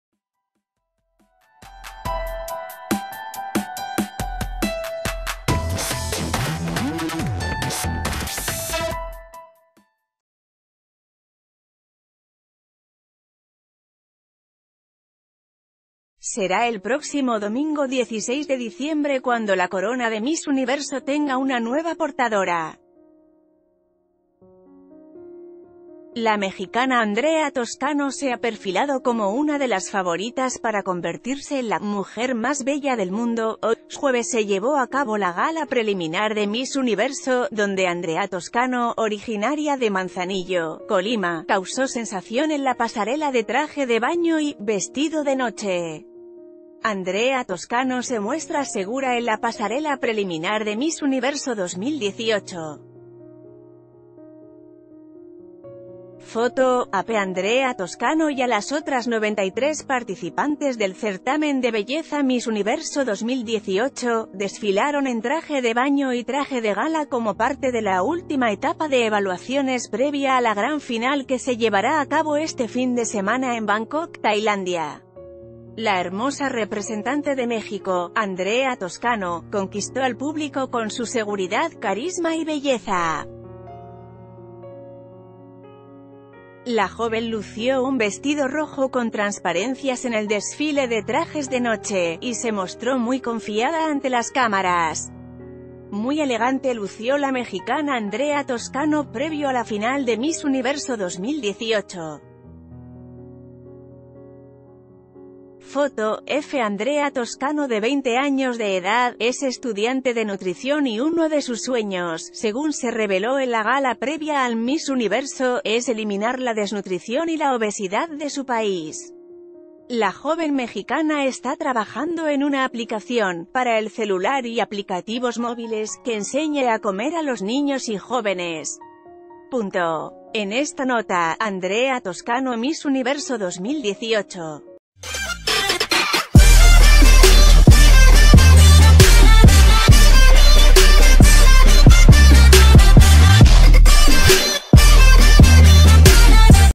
Andrea Toscano entre las favoritas para ganar Miss Universo
Será el próximo domingo 16 de diciembre cuando la corona de Miss Universo tenga una nueva portadora. La mexicana Andrea Toscano se ha perfilado como una de las favoritas para convertirse en la "Mujer más bella del mundo".Hoy jueves se llevó a cabo la gala preliminar de Miss Universo, donde Andrea To...